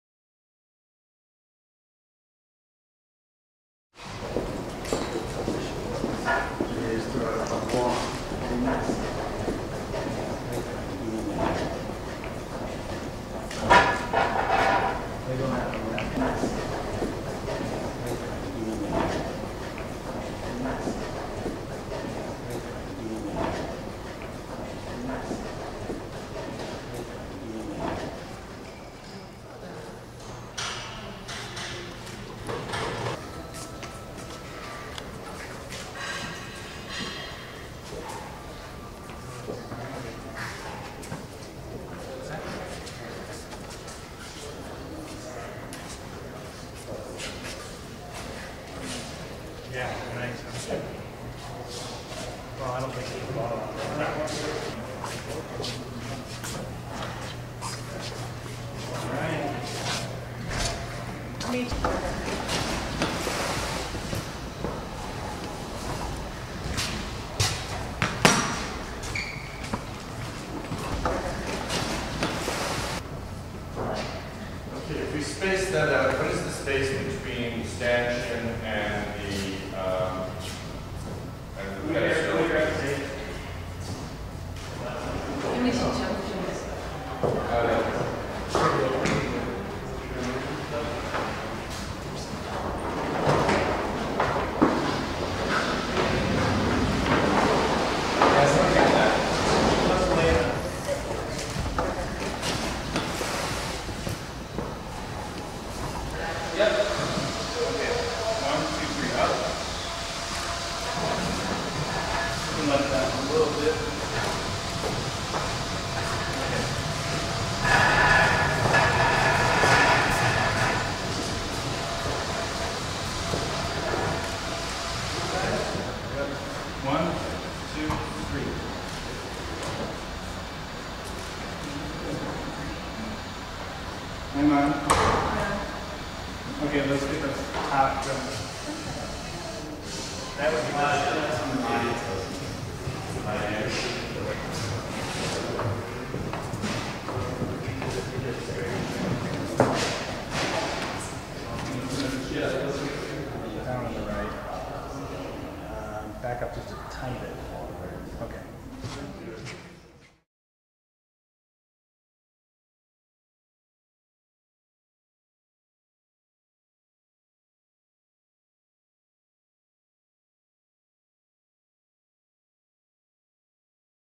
Art Institute Installations: Matisse's Bathers by a River
Watch as Henri Matisse's monumental Bathers by a River is installed in the Art Institute's new Modern Wing. The entire third floor of the new building is dedicated to the museum's collection of modern European art.